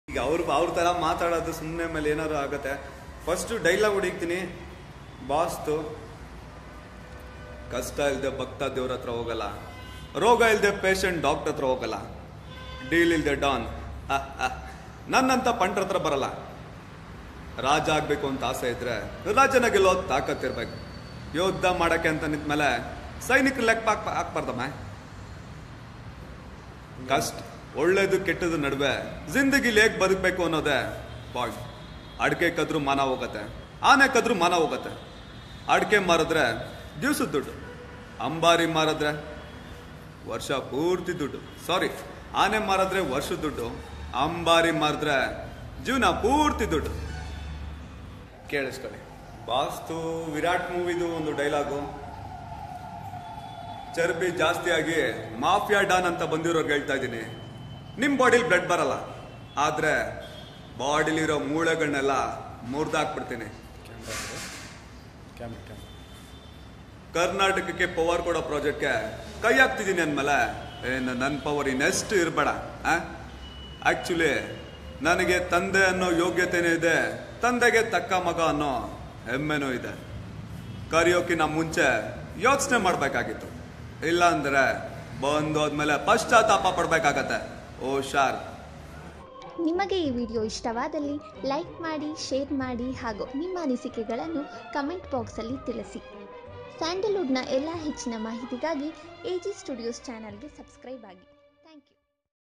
ದರ್ಶನ್ ಧ್ವನಿಯಲ್ಲೆ ಮಾತನಾಡಿದ ದರ್ಶನ್ ಅಭಿಮಾನಿ ನೋಡಿ ಒಮ್ಮೆ Exclusive Video KFI Updates
Darshan Voice by his Fan